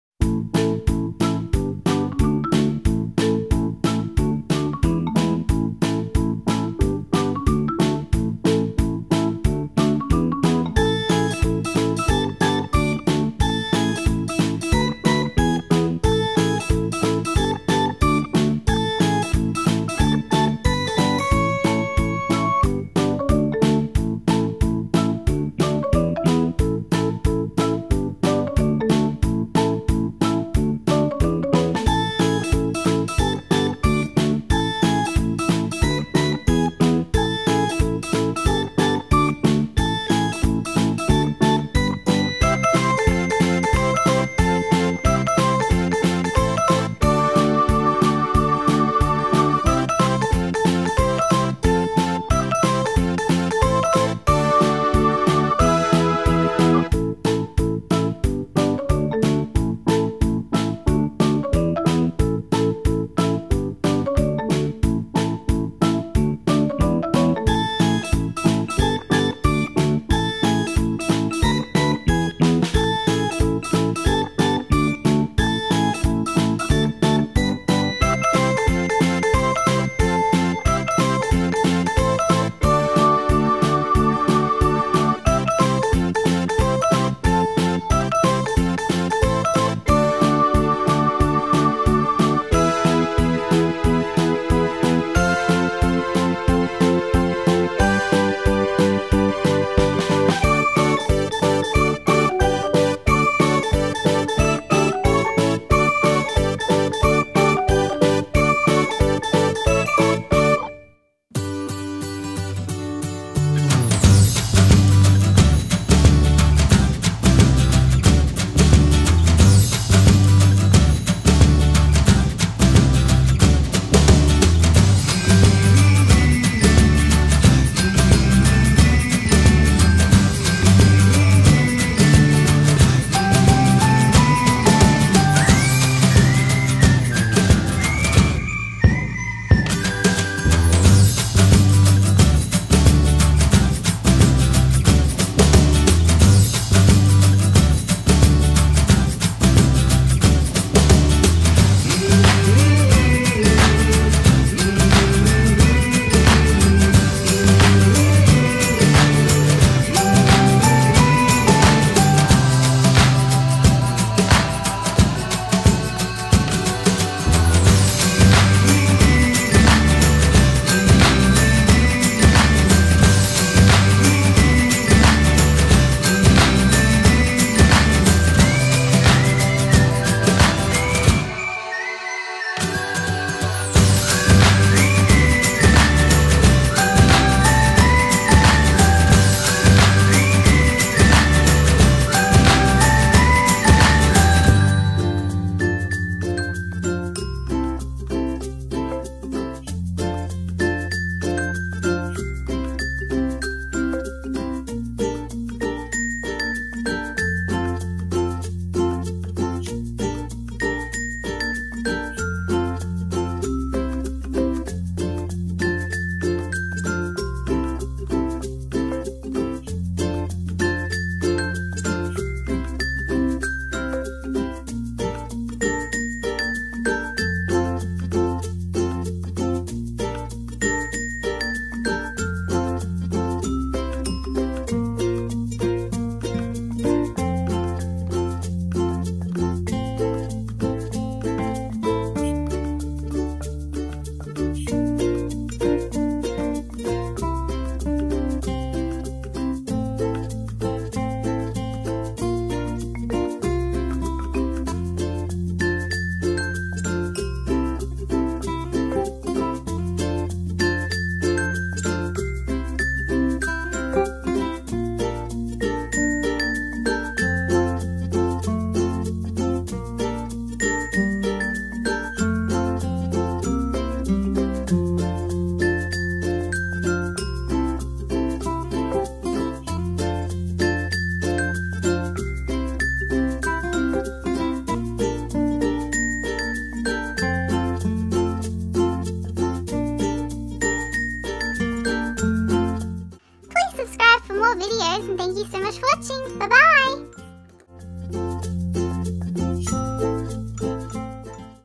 Play Doh Fun with Rainbow Caterpillar and fun cutters
Play and Learn with Play Doh, Fun and Creative video for children. Modelling Clay Molds. Play Dough Play and Learn with molds .Play Doh videos.